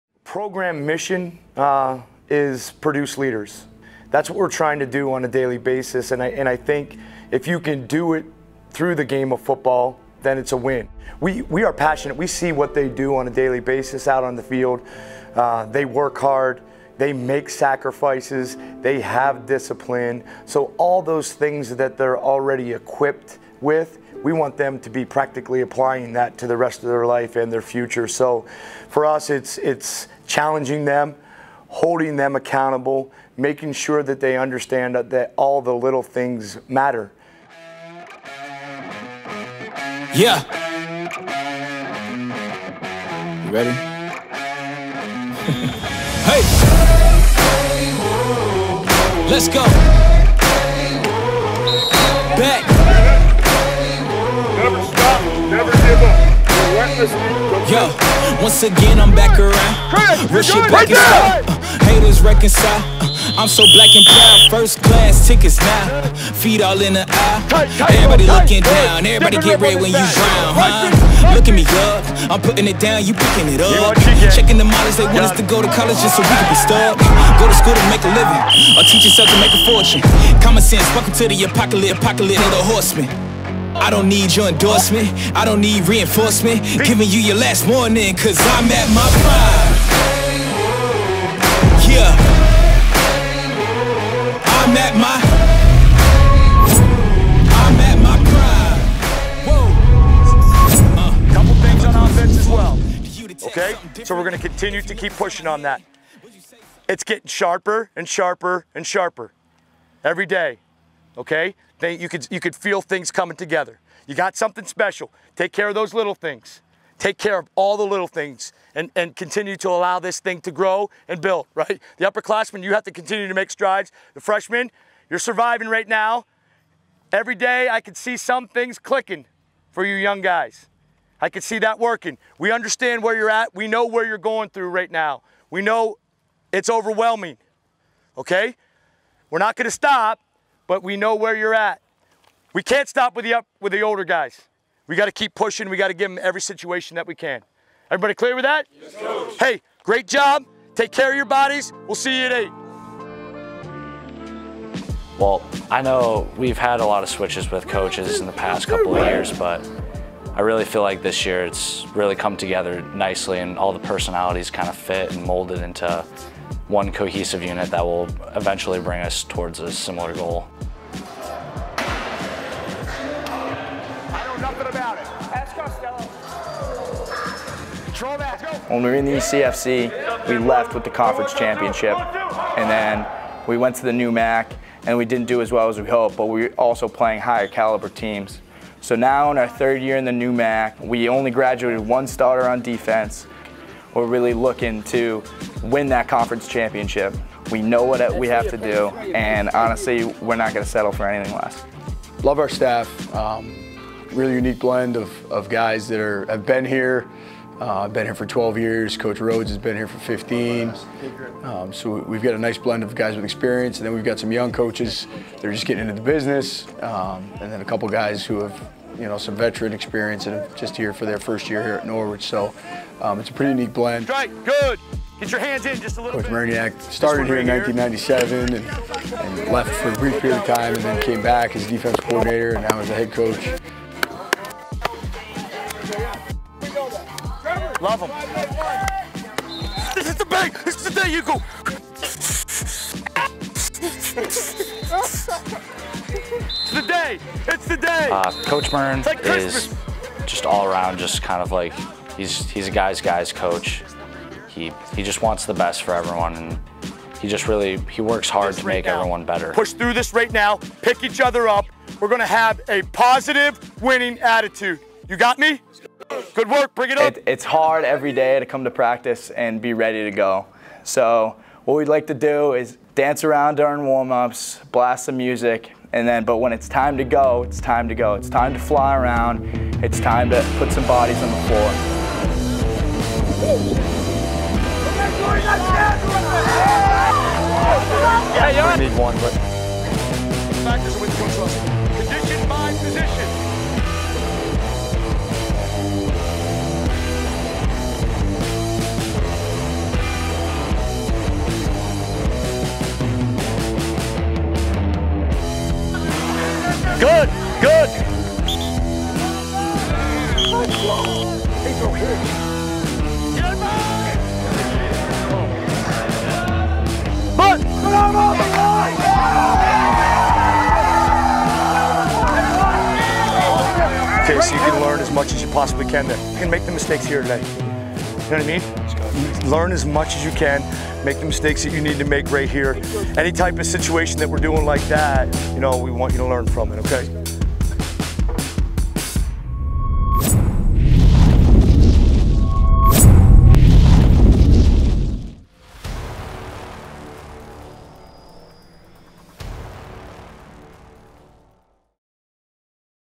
BEHIND THE SHIELD EP:1 with the Norwich University Cadets
Norwich football started camp August 14th with two-a-day practices and team meetings daily. Recently the Cadets scrimmaged Plymouth State University and their season opener takes place on September 7th at Salve Regina.

Mark your calendars for the home opener at Sabine Field on September 14th, they take on St. Lawrence at 12:00 PM.

“Behind the Shield” is a new exclusive series produced by Ronny DiMasi featuring a behind the scenes look at Norwich University football. From training camp to the regular season, we'll be following the action every step of the way.